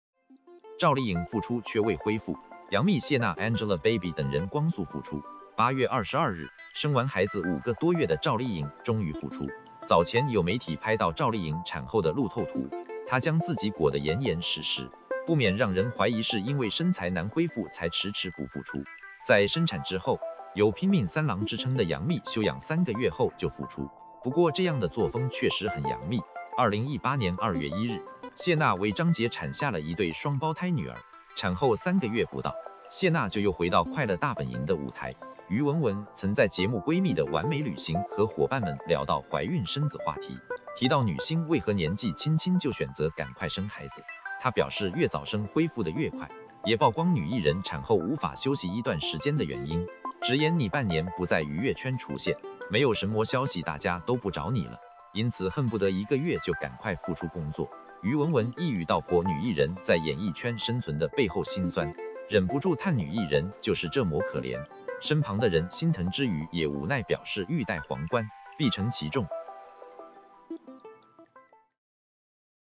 赵丽颖复出却未恢复,杨幂谢娜angelababy等人光速复出
➤Thanks收看！